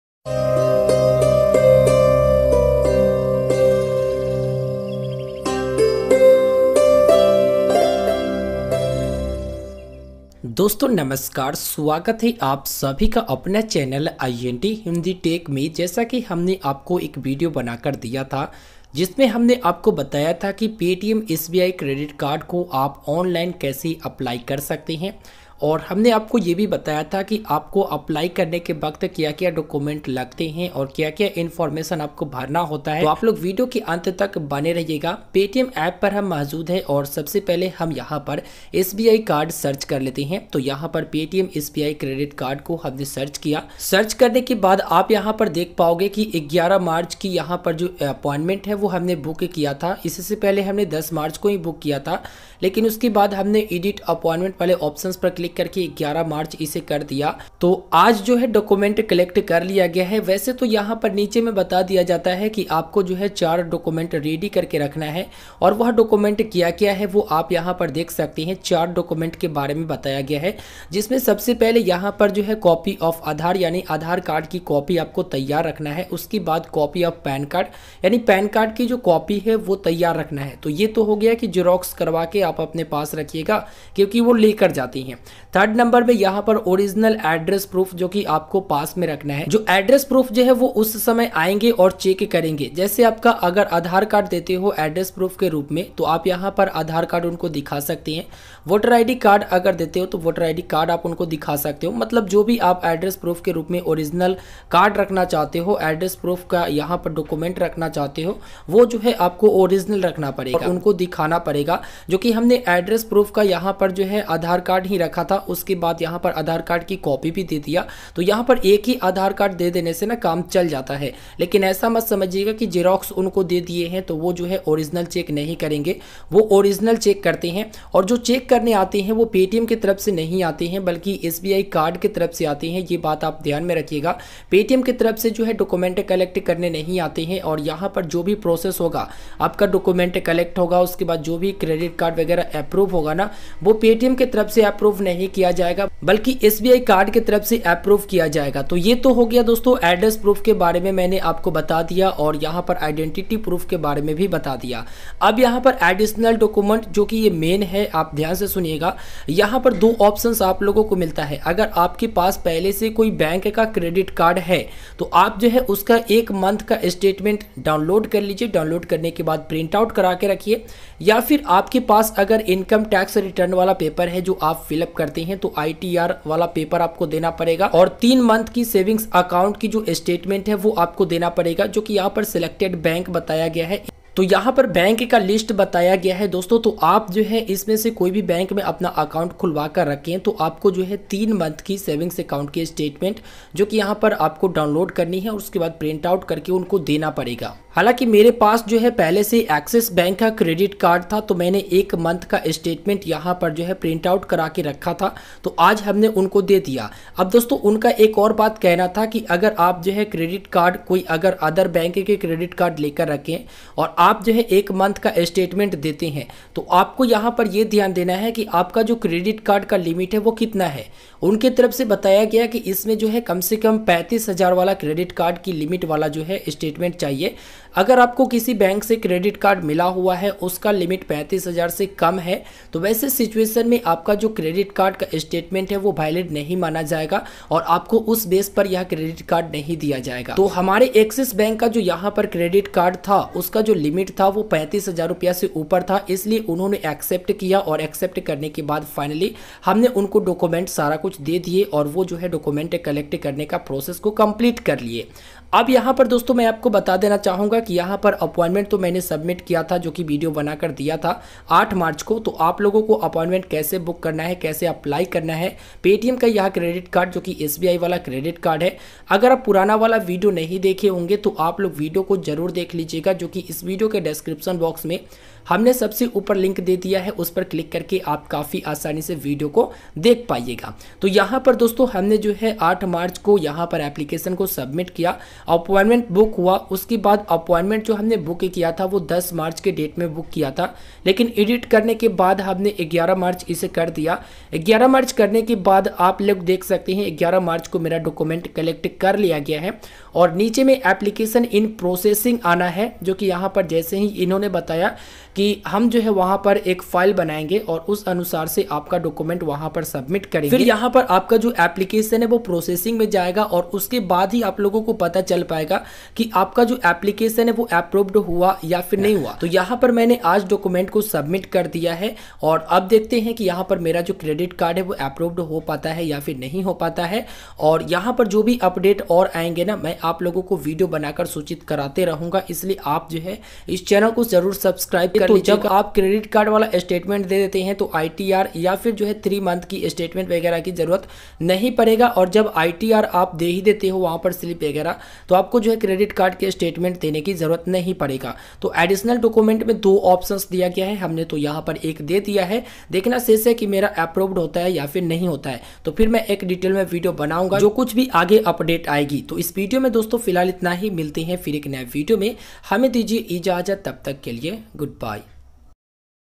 Paytm sbi credit card documents collect 🤔| paytm sbi card eligibility
Dosto aaj ke is video me humne bataya hai paytm sbi credit card me lagne wali documents ke bare me... Aap video ko ant tak tak jarur dekhe...
Thanks🌹🙏🙏

dhanyavaad...!

Help & Support.. Join Telegram channel for instant details..